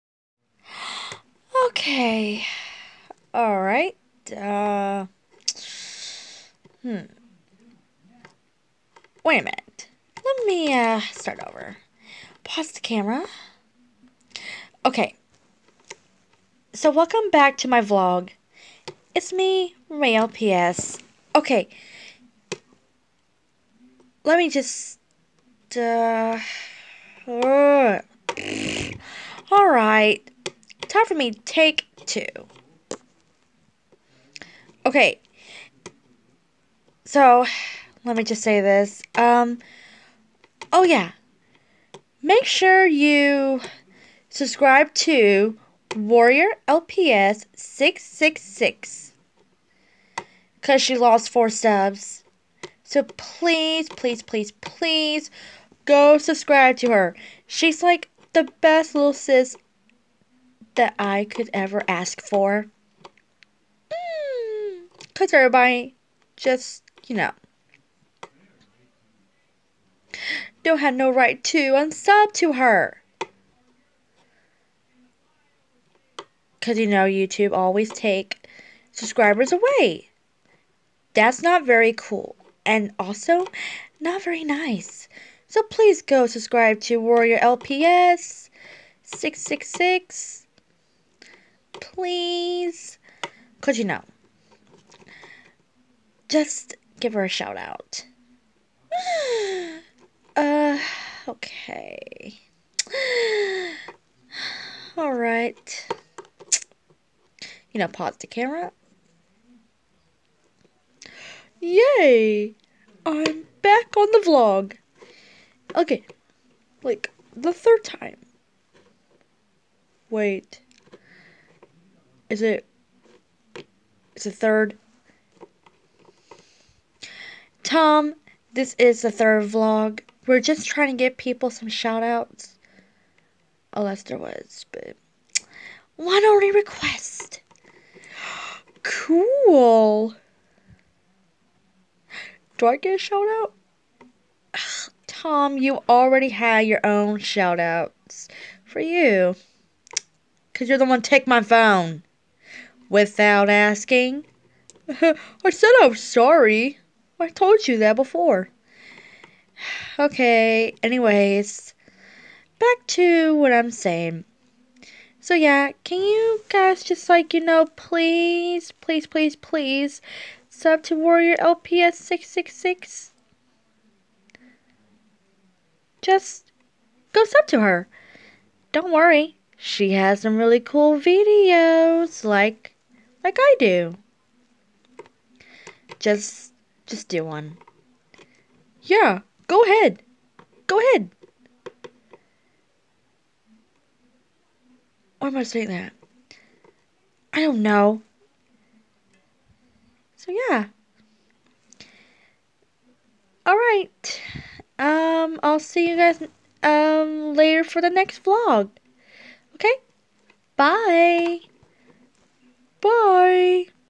Vlog #7: Sub To Warrior Lps666
Hey my Ray Ray LPS fans,
She just lost 4 subs and I hope you guys sub to her. See you guys in my next vlog!!! See ya!!!
~Ray~